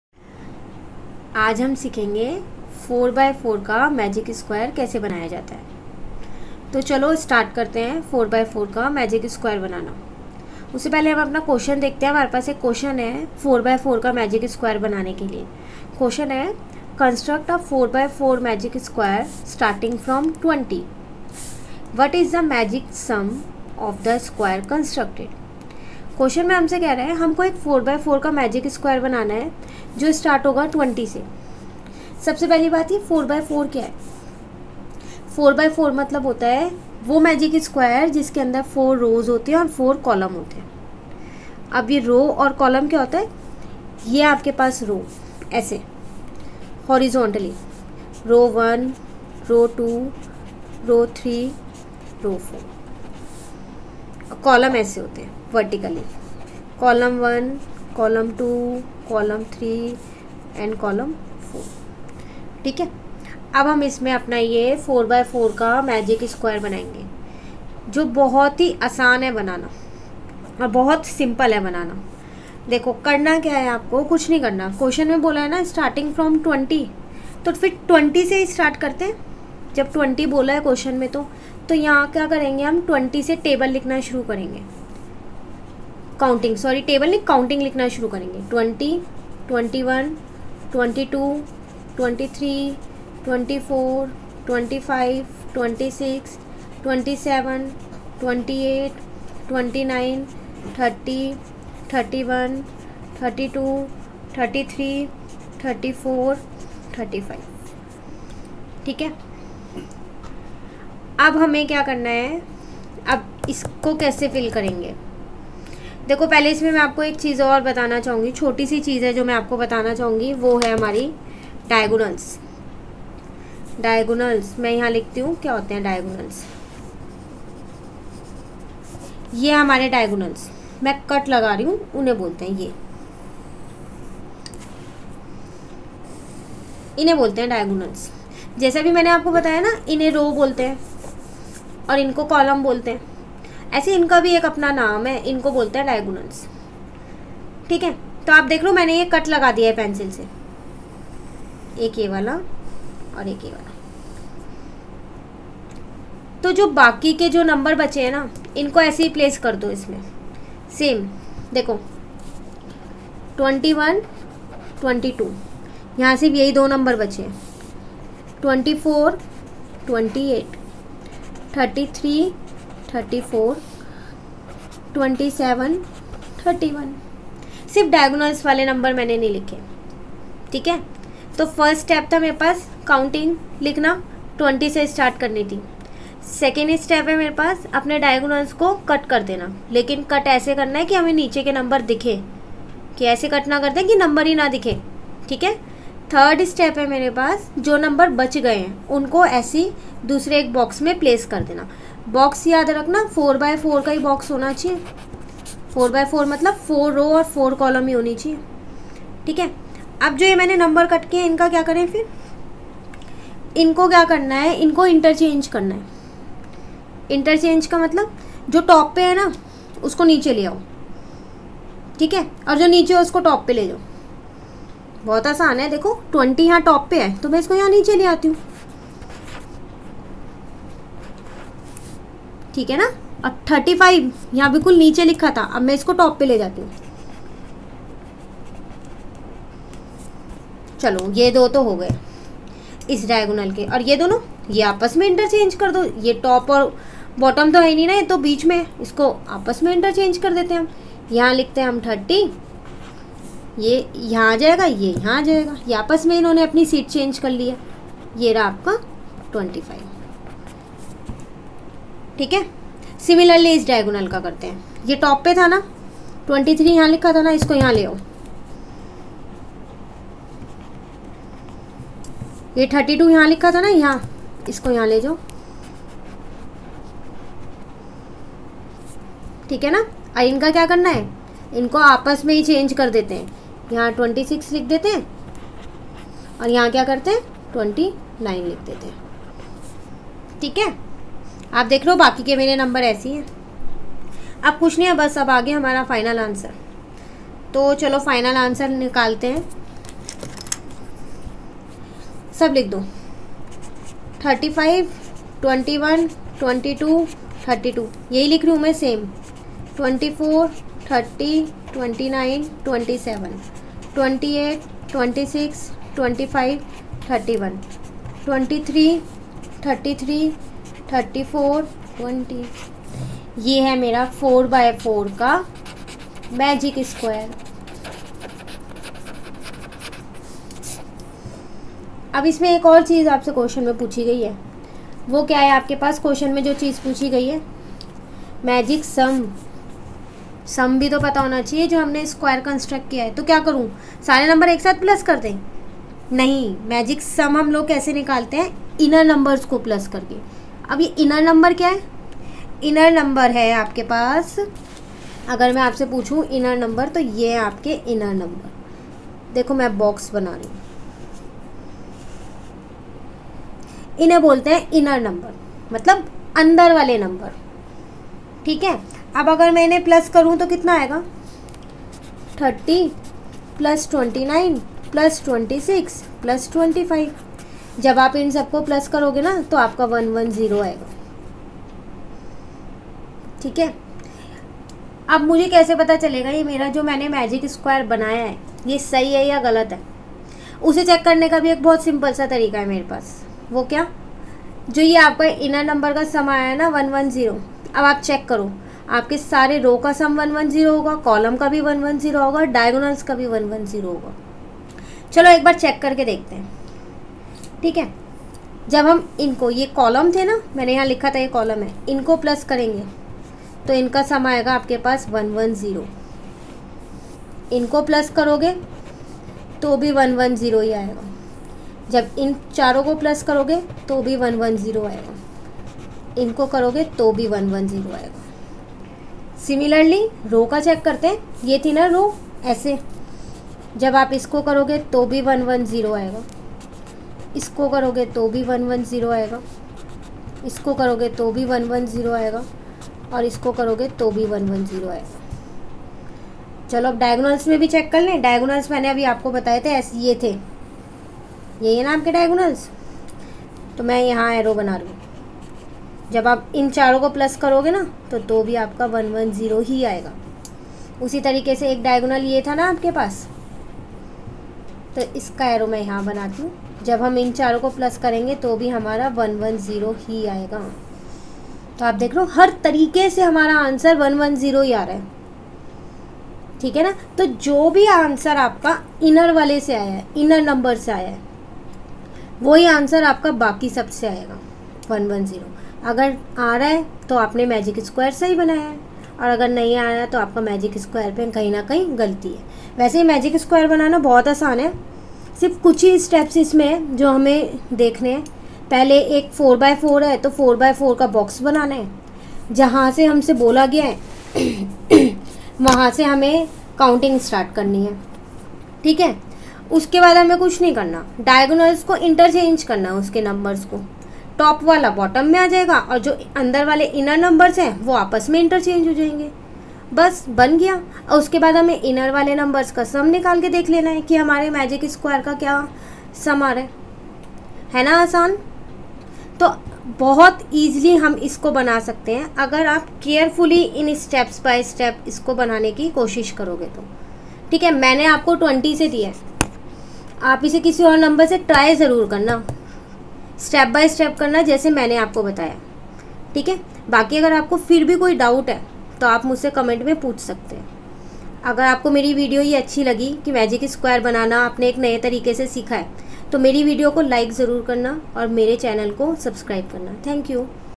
4x4 magic square | 4 by 4 magic Square | Maths magic tricks
magic square 4x4 | 4 by 4 magic Square | magic square | magic square 4x4 | Maths magic tricks
In this video I am going to explain, how to construct 4x4 magic square with simple method.